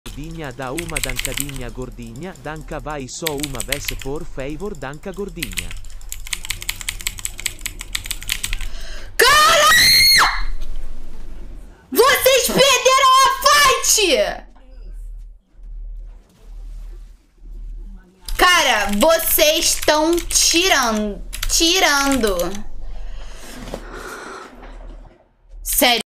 YULLA FULL PISTOLA FAZENDO 2V1 ENQUANTO O TIME OLHA KKKKKK
👑 Quer subir de elo muito mais rápido através de aulas exclusivas com pro-players?
👑 Então acesse e VEM SUBIR DE ELO COM A GENTE!